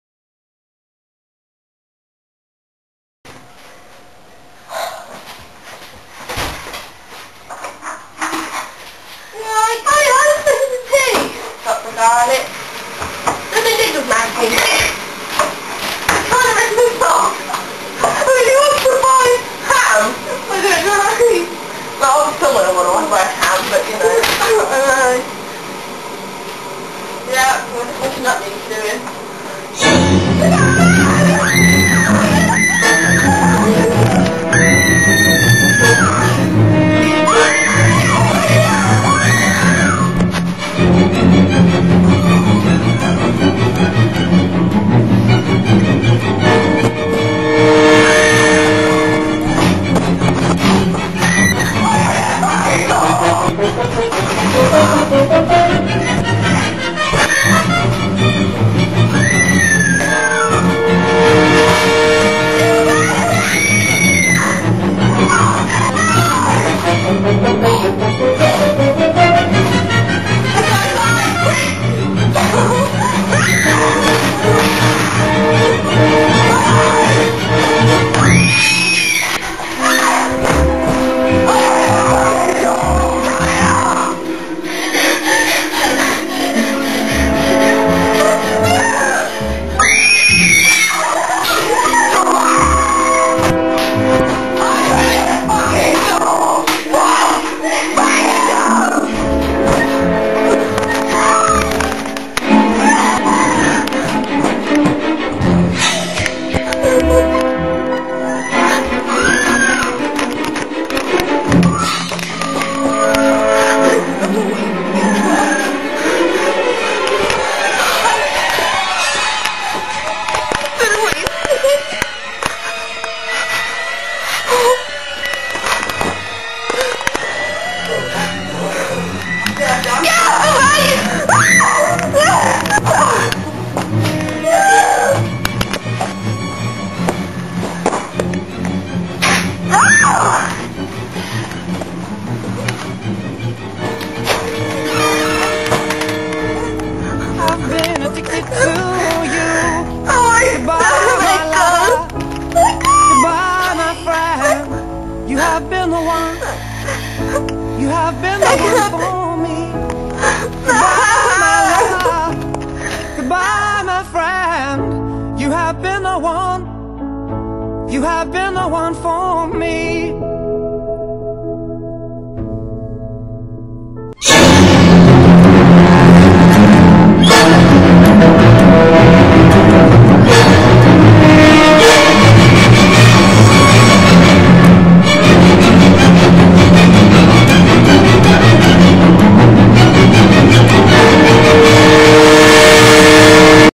Lock Your Doors
Megan, Jayde And Steph Made Our Own Horror Clip Enjoy !!